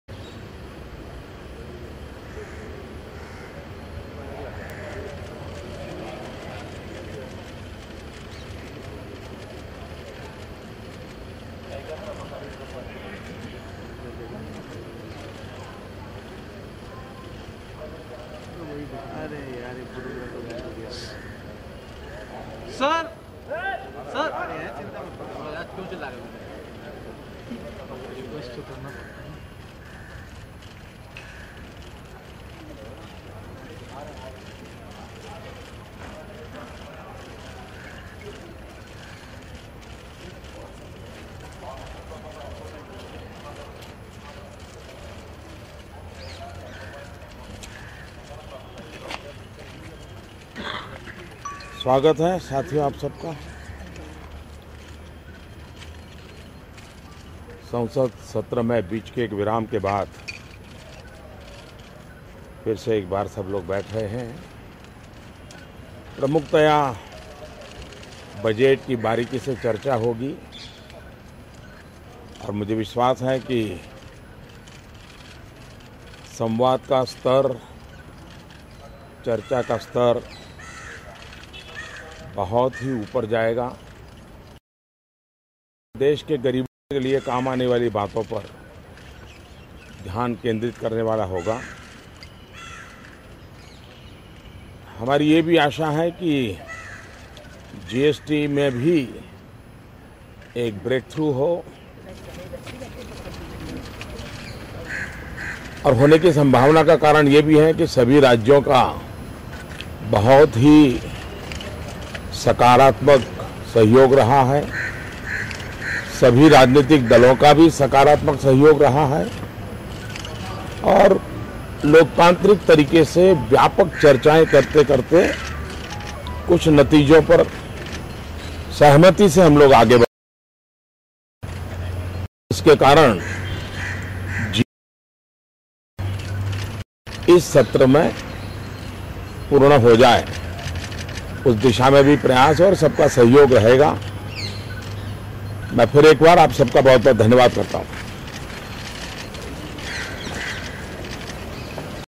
PM Modi's statement to the media ahead of the Parliament Session
Prime Minister Narendra Modi's statement to the media ahead of the Parliament Session